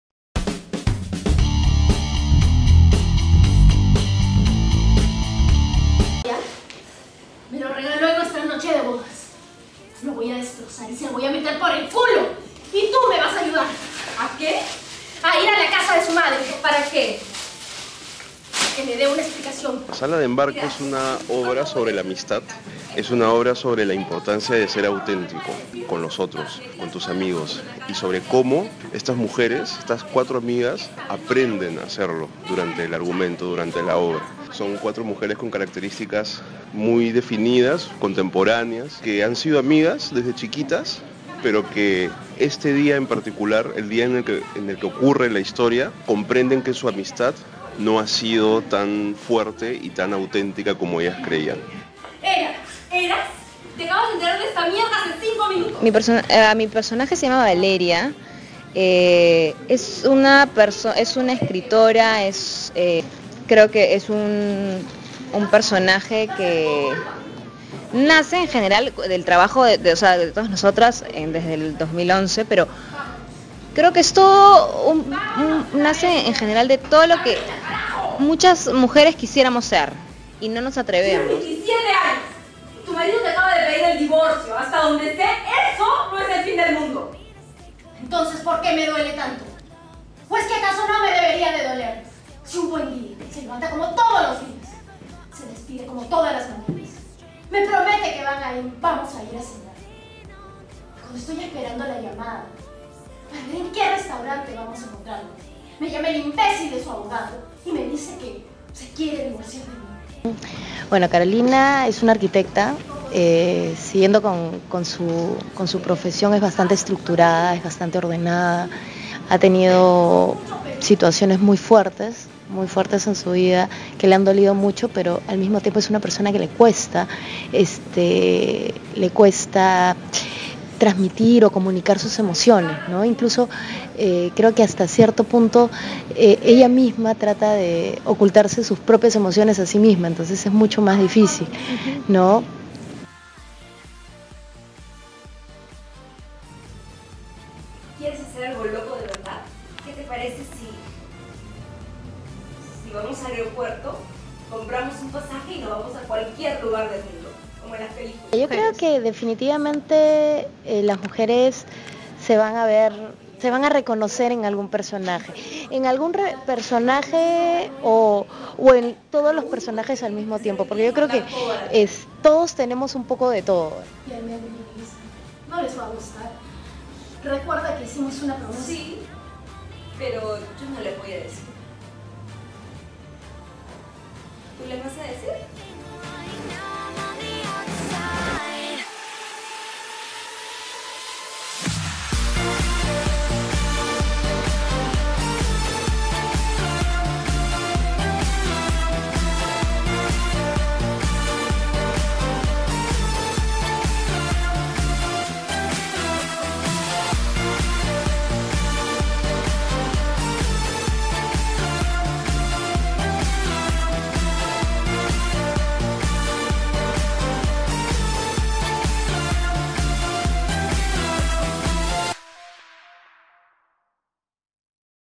“Sala de Embarque”: Una prueba de amistad
Obra teatral producida por Rara Maquinaria en el teatro del Centro Cultural Cafae-SE
SALA DE EMBARQUE
Temporada:  Del 13 de marzo hasta el domingo 10 de mayo. 2015
Horario: Viernes y sábados a las 8 p.m. Domingos a las 7:30 p.m.
Lugar: Centro Cultural CAFAE-SE (Av. Arequipa 2985, San Isidro).
Entradas: boletería del centro cultural y en Atrapalo.pe desde la segunda semana de temporada.
Apta para mayores de 13 años de edad.
Elenco: Rocío Olivera (Almendra), Angie Ruiz (Carolina), Suzetti Hananel (Julissa) y Cecilia Silva (Valeria).
Precios: General: S/. 20 / Estudiantes y jubilados: S/. 15 / Activos y Cesantes del Sector Educación: S/. 6